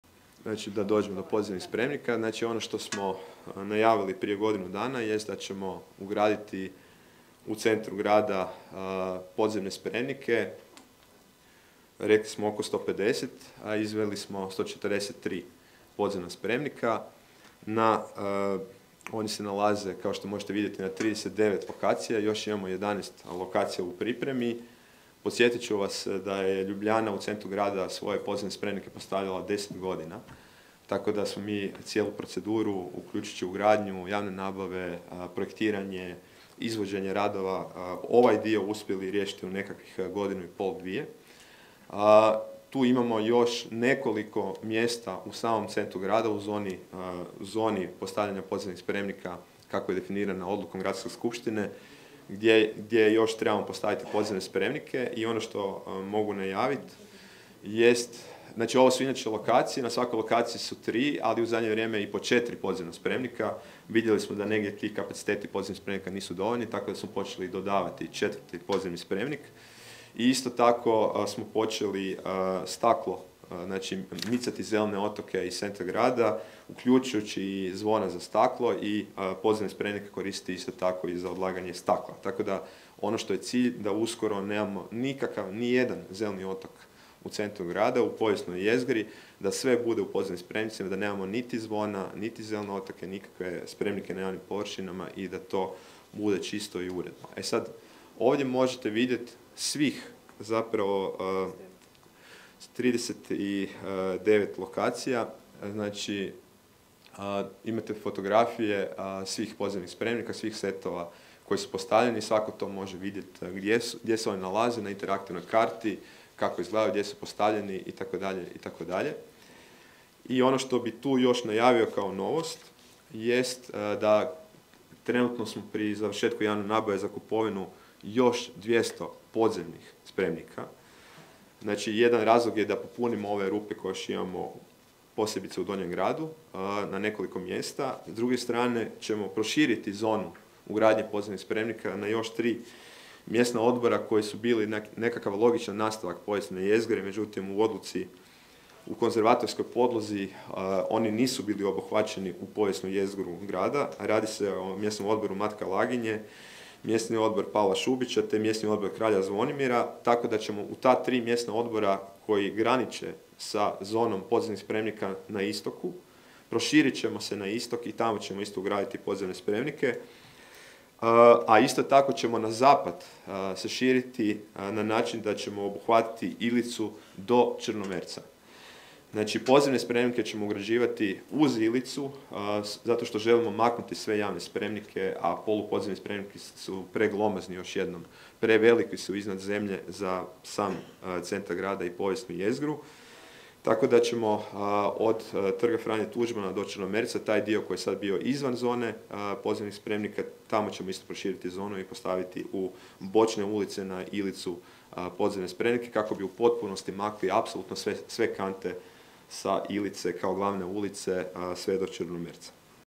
Tomislav Tomašević o ugradnji podzemnih spremnika u centru Zagreba
Gradonačelnik Grada Zagreba Tomislav Tomašević i zamjenici gradonačelnika Danijela Dolenec i Luka Korlaet, uz prisustvo direktora zagrebacke čistoće Davora Vića održali su konferenciju za medije na temu novosti vezanih uz model sakupljanja otpada.